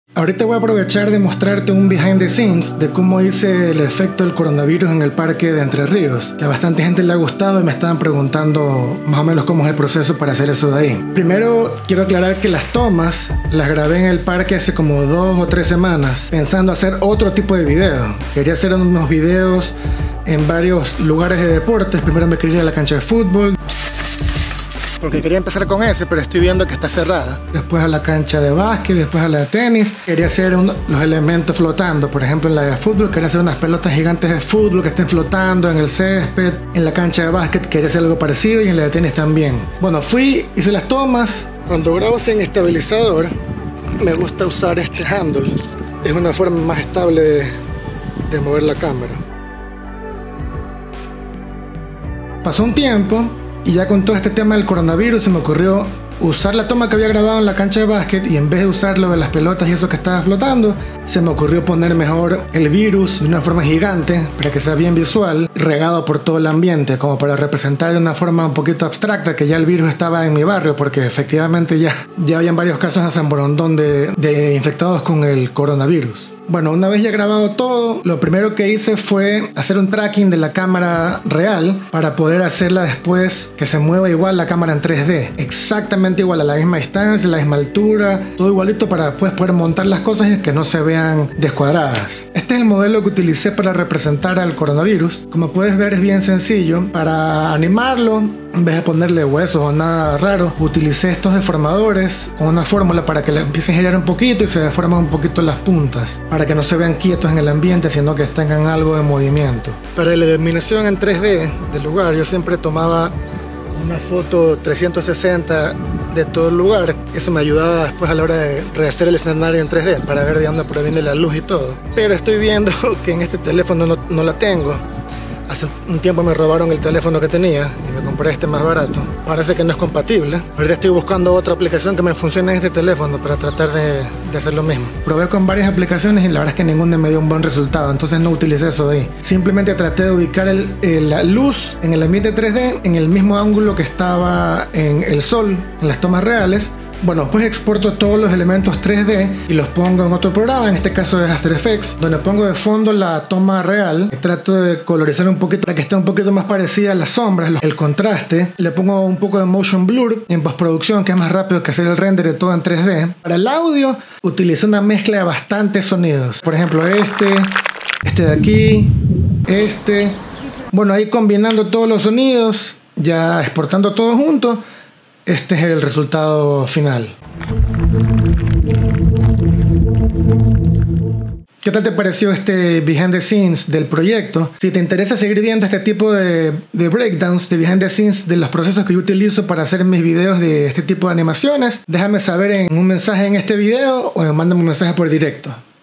Coronavirus en Samborondón (CG) Behind the Scenes (vertical video)
Este es el behind the scenes del proceso de realización de mi video del coronavirus en samborondón, utilizando tomas reales y elementos 3D para montar en el ambiente de una forma bien abstracta el virus. Disculpen que sea en vertical el video pero lo hice pensando sobre todo en mi canal de IGTV.